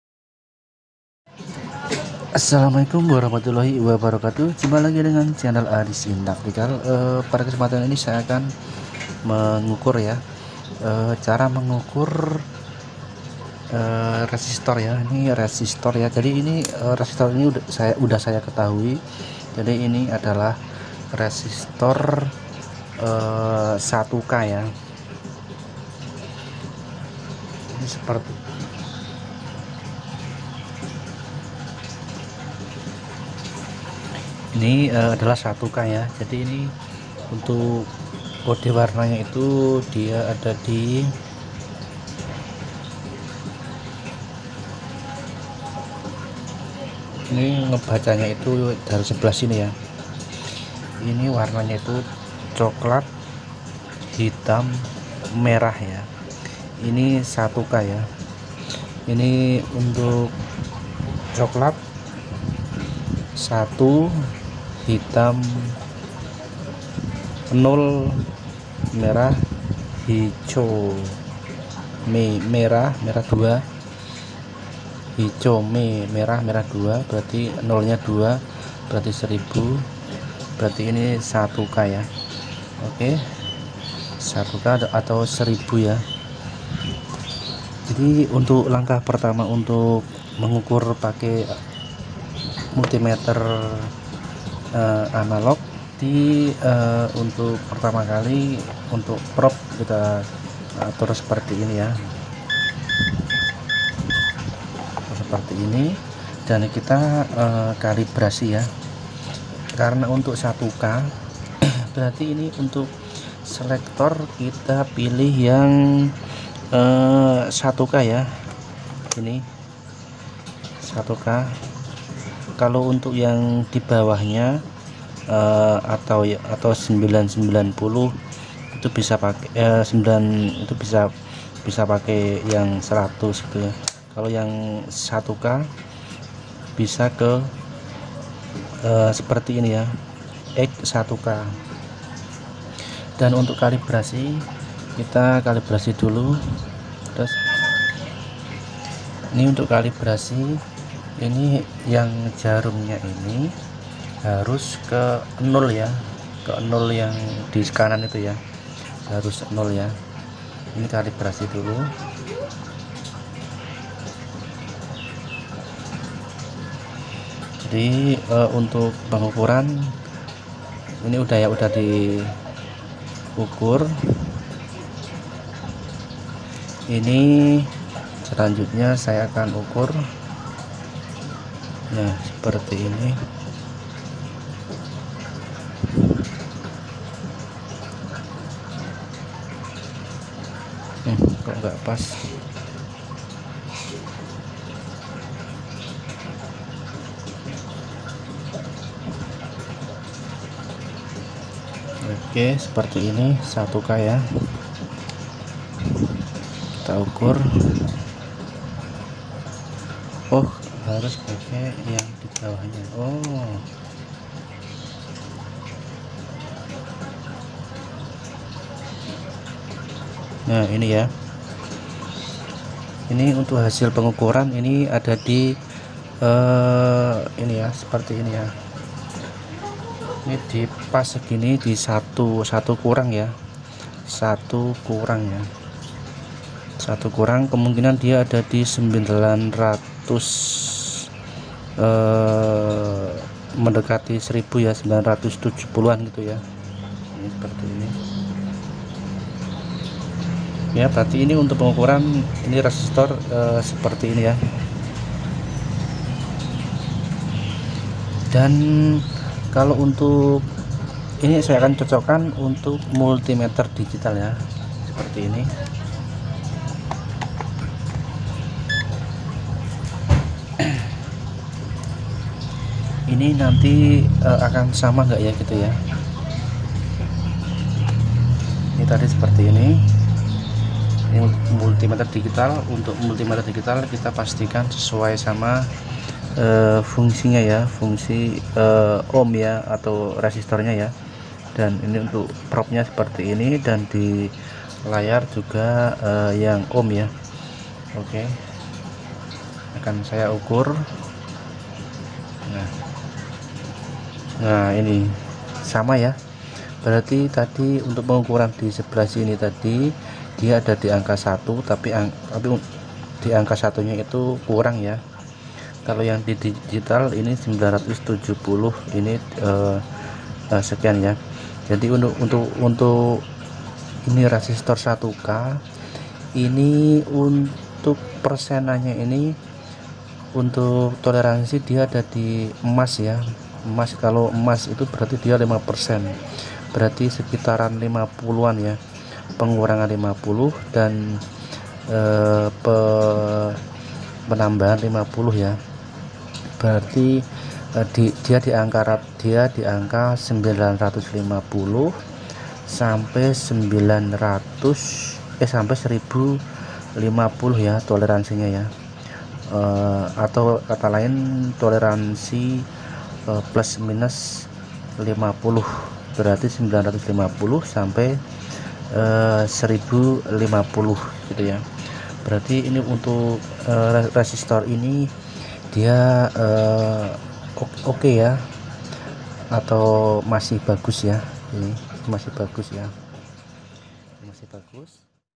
Cara mengukur Resistor menggunakan multimeter digital dan analog
inilah Cara mengukur Resistor menggunakan multimeter digital dan analog